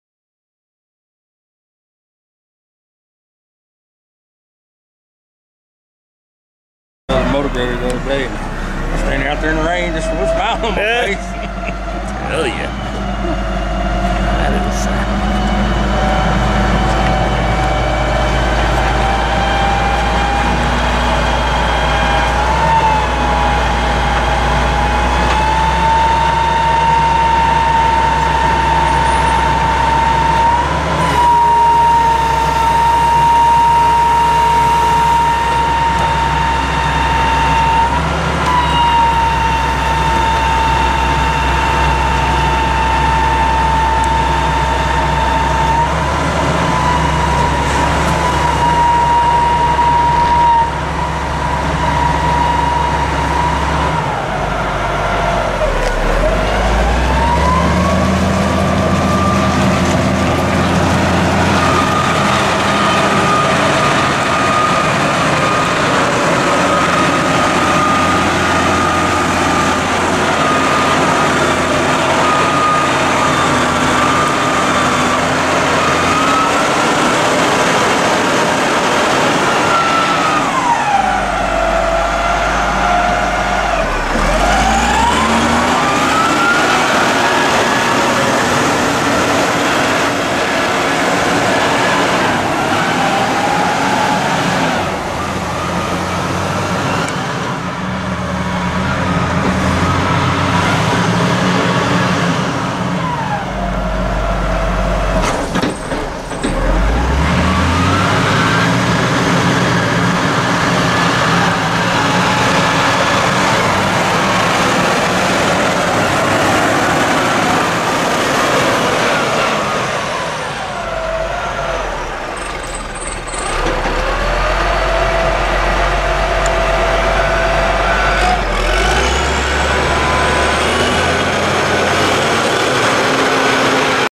1999 Champion 736A motor grader for sale | sold at auction August 15, 2013
1999 Champion 736A motor grader
10,462 hours on meter
Hour meter inoperable
Estimated 11,000 total hours
Cummins 6C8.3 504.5 CID six cylinder diesel engine
215 HP
Fully sequential direct drive powershift transmission
8F-4R gears
All wheel drive
Enclosed cab
AC and heat
AM/FM
Suspension seat
Front and rear work lights
16&#039; moldboard
Tip control
Side shift
Hydraulic pump has been rebuilt
14.00x24 tires
34,940 lbs. base operating weight
Serial 029846

It was located in Hamlin, TX, and we sold it to the highest bidder for $35,750.